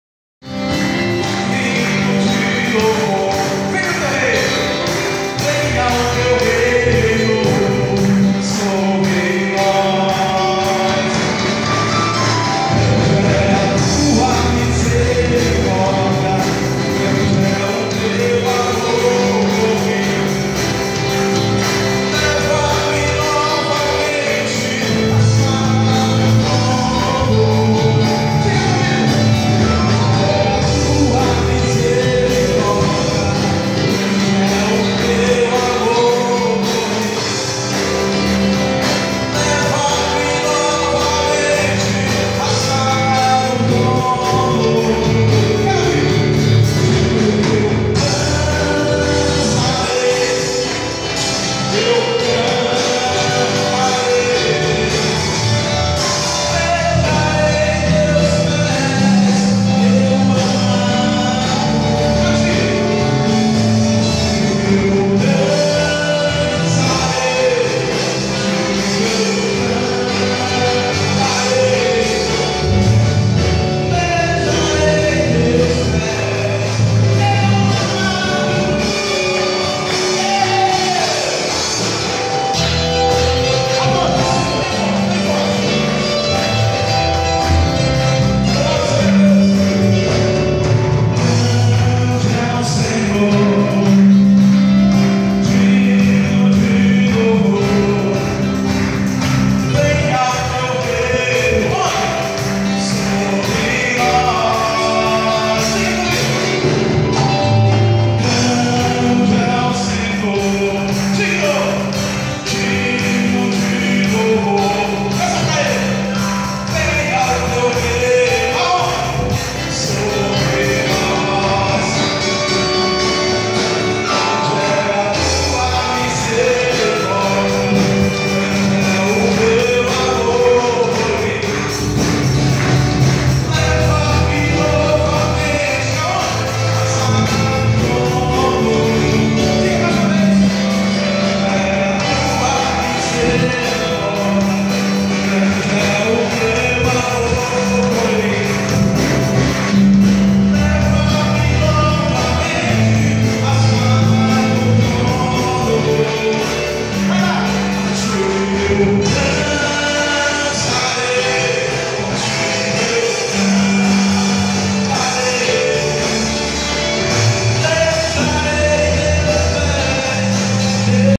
Adorando o Senhor na igreja Madureira (Araguaína-TO)
Culto abençoado na direção da mocidade, na igreja madureira em Araguaína - TO onde o Senhor se fez presente poderosamente.